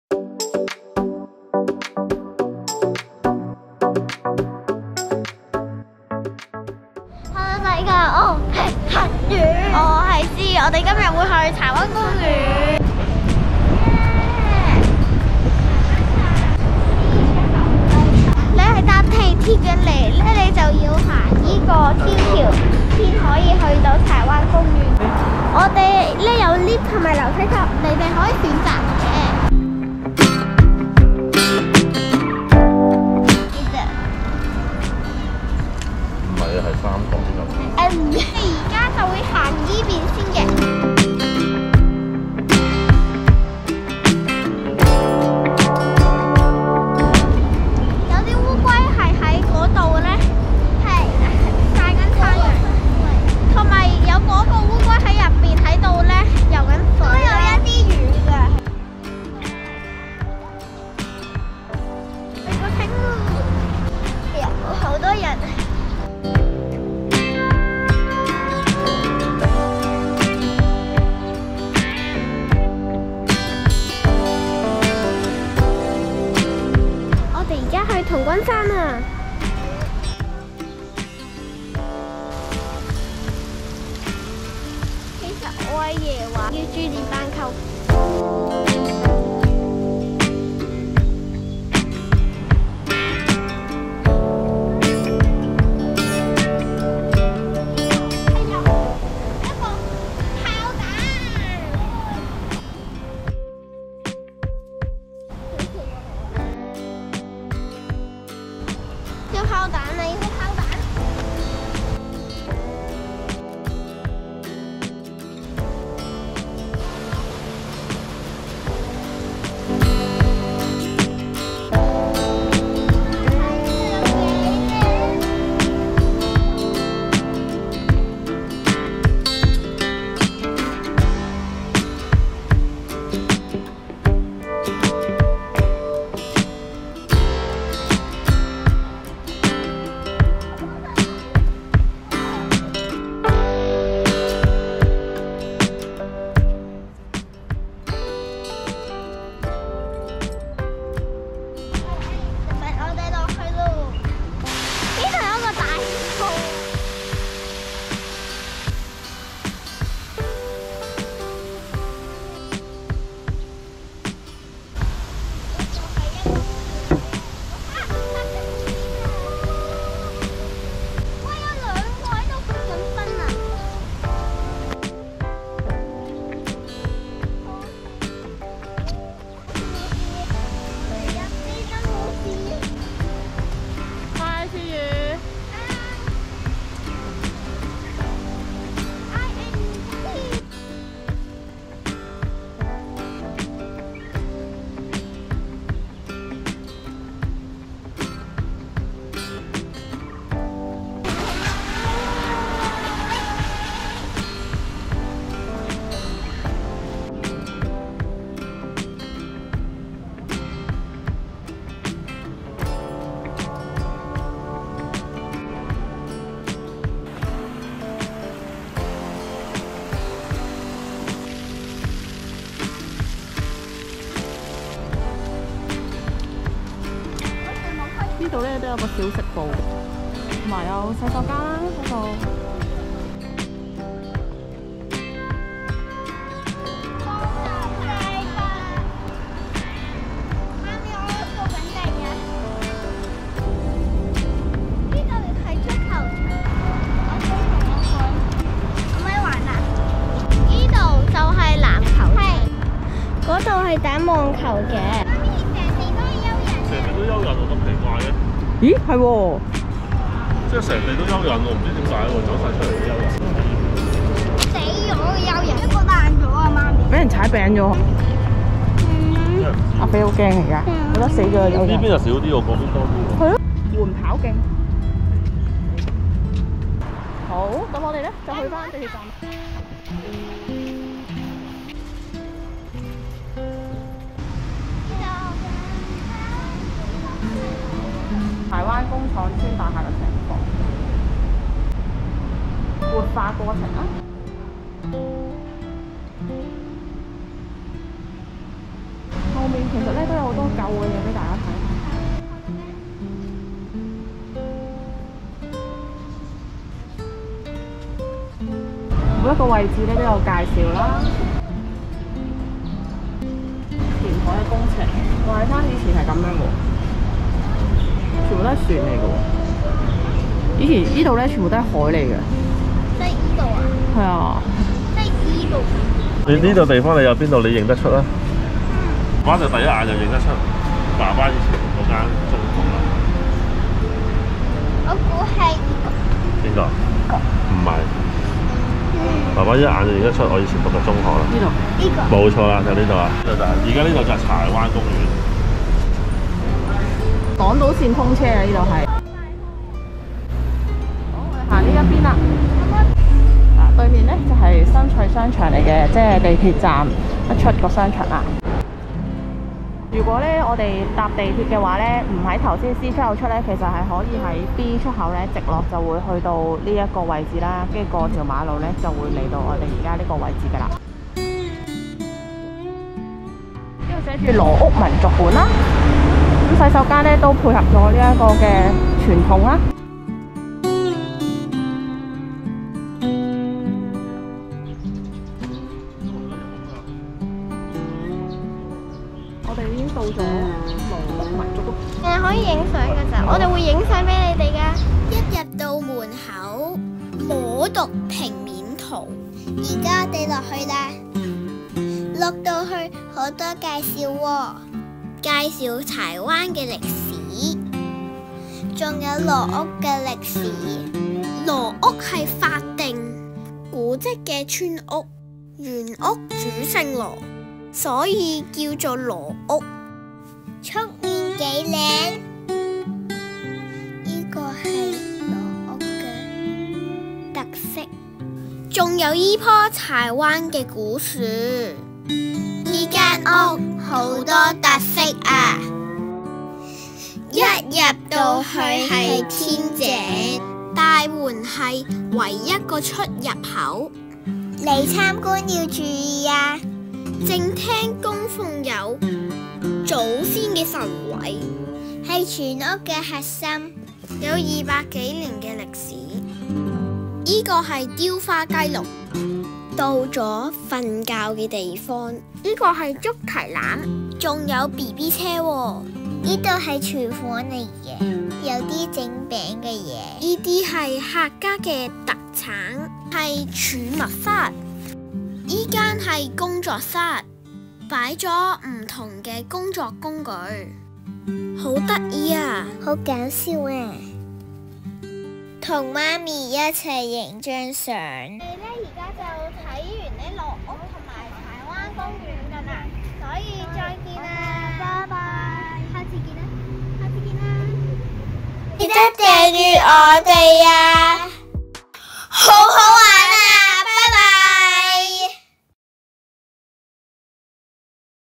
【童遊柴灣】柴灣公園｜看烏龜🐢 地上好多蚯蚓｜二百多年歴史的法定古蹟村屋| 柴灣民俗館｜了解柴灣發展｜柴灣工廠邨大廈｜親子遊 ｜香港半天遊
放假做咩好，周圍去玩下睇下食下😝
今次去了柴灣公園，柴灣民俗館，仲有柴灣工廠邨大廈，玩之餘仲可以了解到柴灣的發展